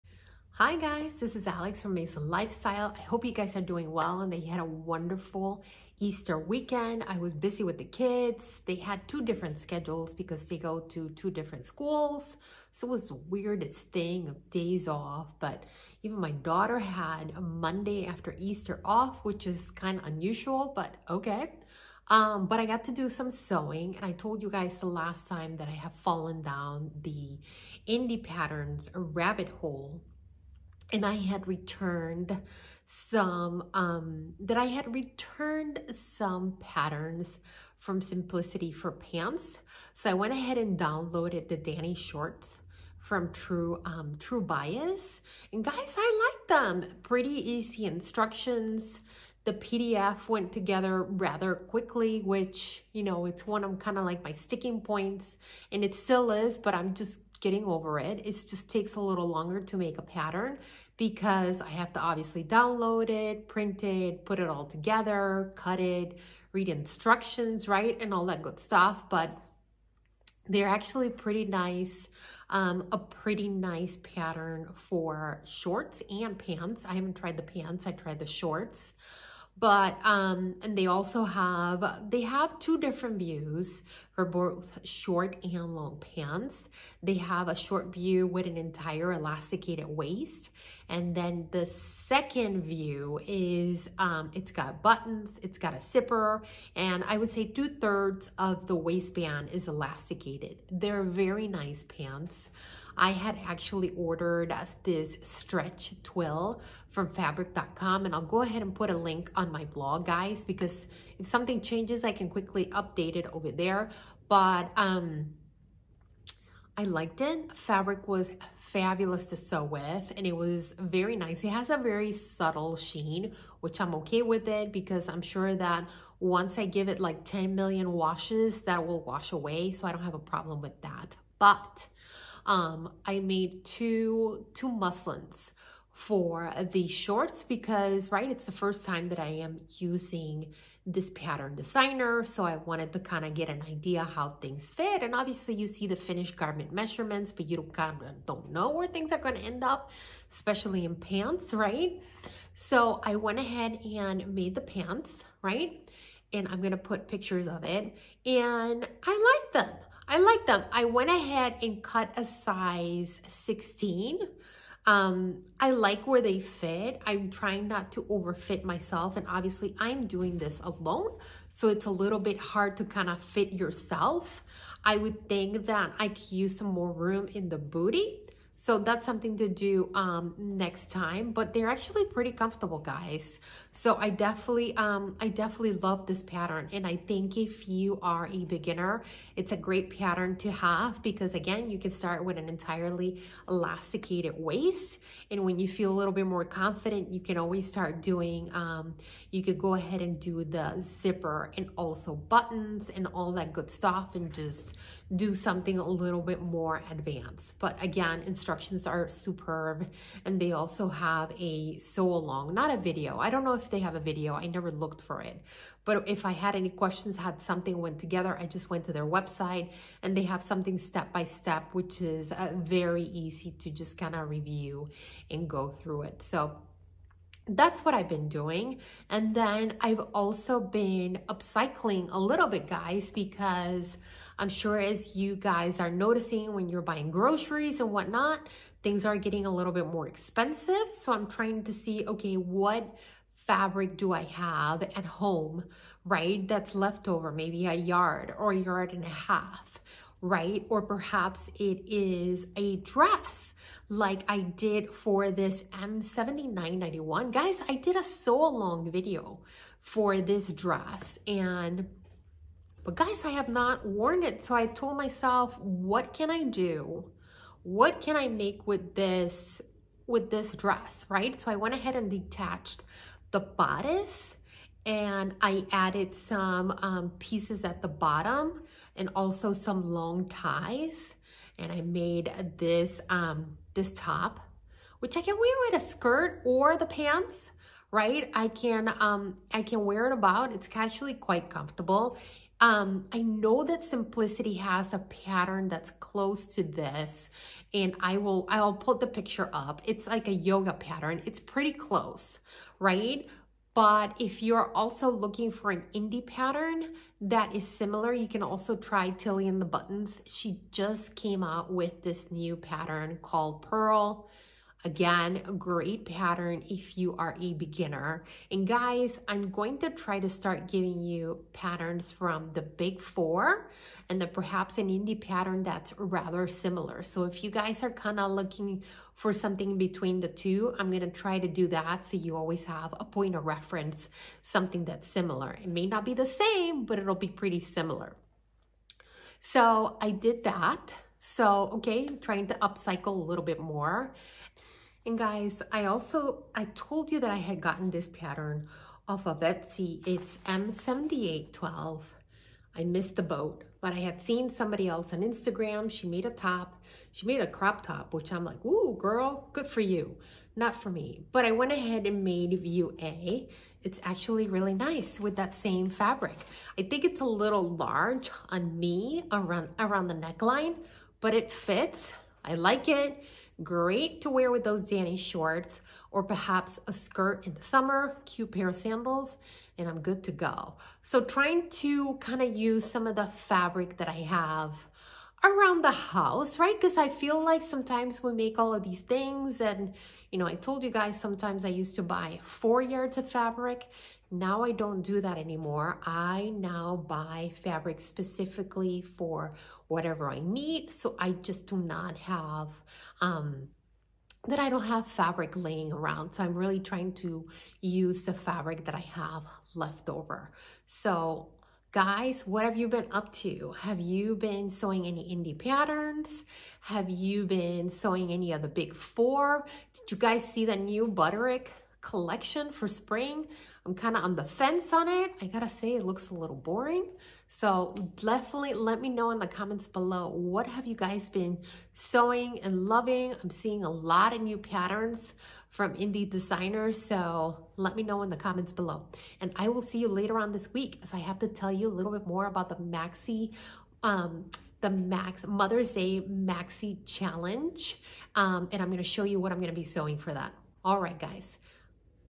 Making the Dani Shorts & Upcycling
I finally made the Dani Shorts by TrueBias (love them!) and I'm also up cycling a dress that I made but have yet to wear.

for links on the where you can get the patterns and fabrics I talked about in this video.

These change (alllll the time) but to give you a reference on my measurements since we are sewing: I am 5’5” and my measurements are as follows, upper bust 44”, full bust 45.5”, waist 39”, hip 46.5”